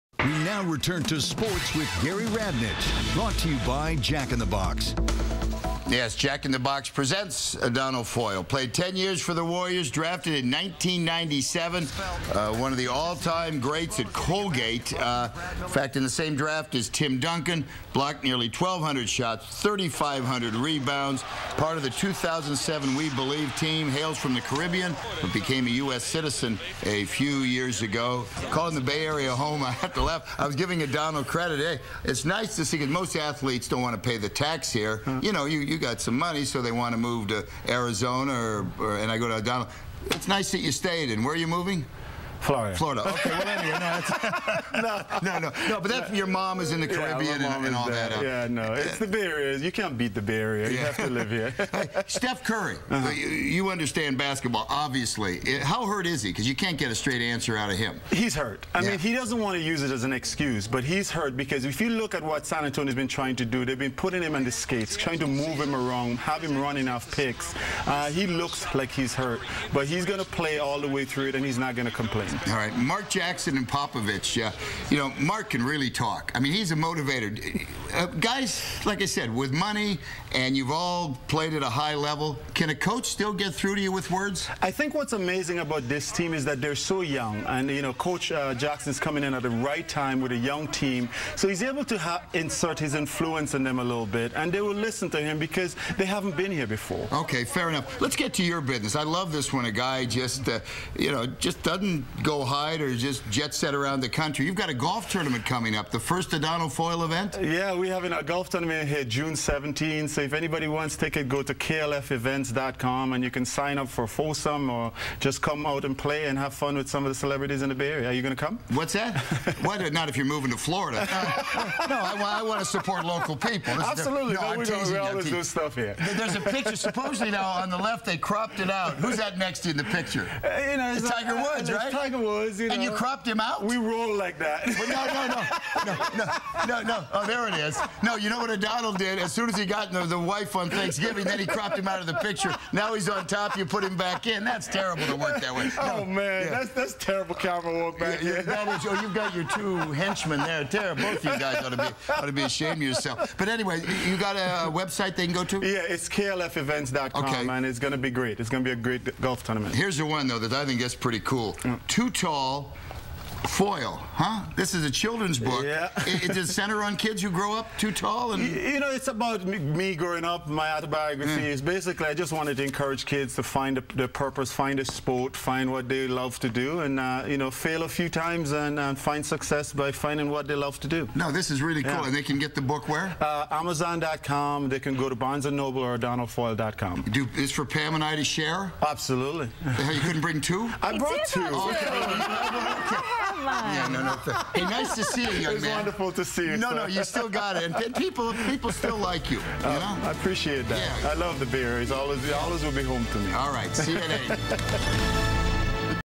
Former Warrior Adonal Foyle Holds Charity Golf Tournament
KRON 4's Gary Radnich